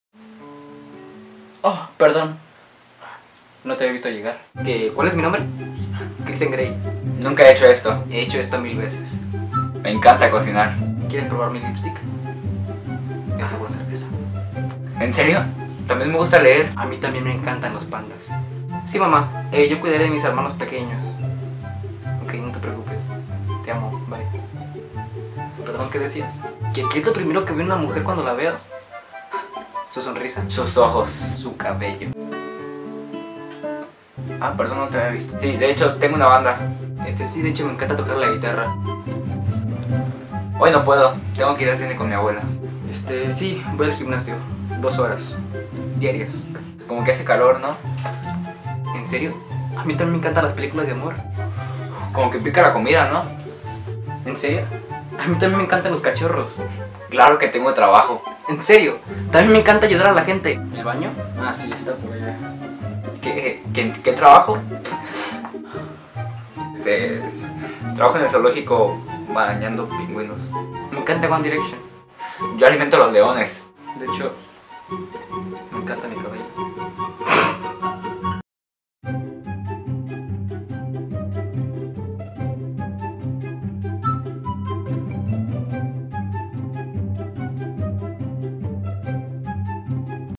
COSAS QUE DICEN LOS HOMBRES PARA IMPRESIONAR A LAS MUJERES.
Algunas de las frases que pueden llegar a decir los hombres al momento de querer impresionar a la mujer que les gusta.